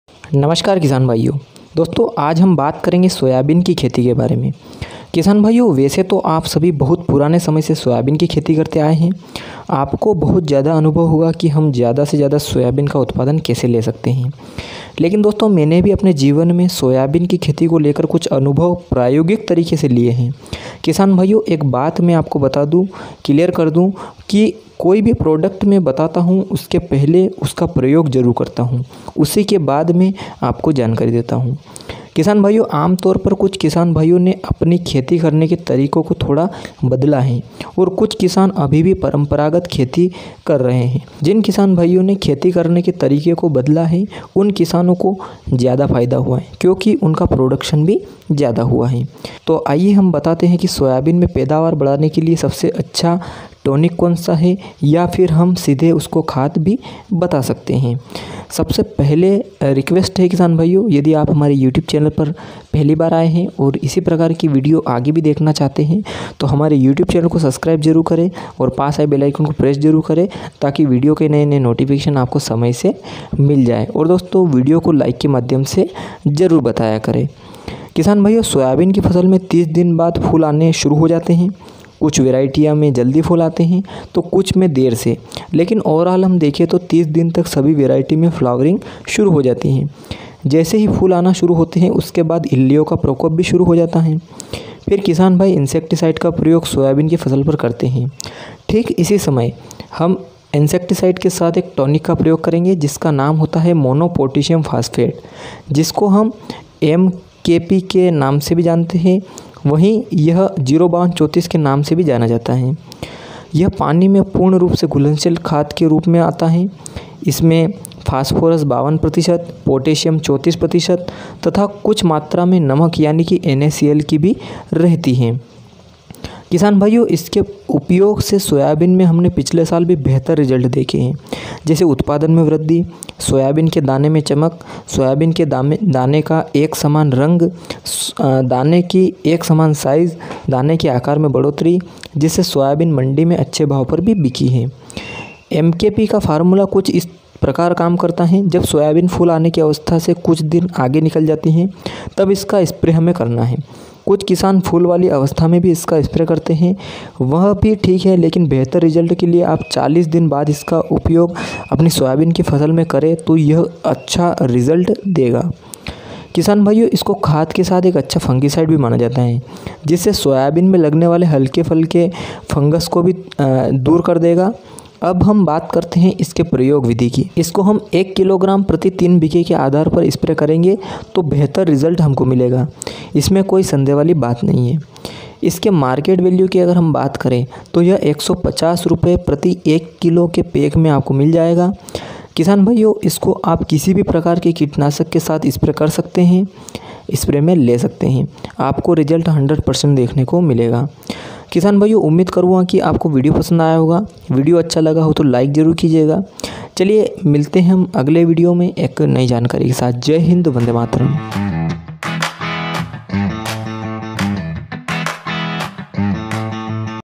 सोयाबीन में 35 दिन बाद करे यह स्प्रे 💦|Soyabean me best khad|सोयाबीन की बम्पर पैदावार इस प्रकार ले
सोयाबीन के लिए सबसे बेस्ट खाद

बेस्ट खाद सोयाबीन की पैदावार बढ़ाने के लिए

बेस्ट टॉनिक फ़ॉर सोयाबीन की फसल